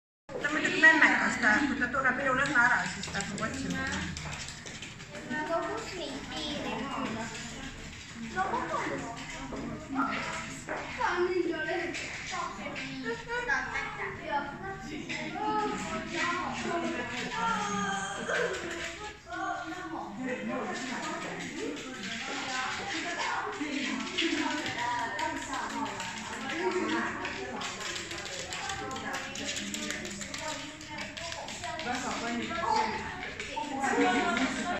Läheneva 1. septembri puhul kutsus Keskkonnaagentuur oma töötajate lastele, kes esmakordselt kooliteed alustavad, külla mõned eksootilised sõbrad Nõmme Loodusmajast.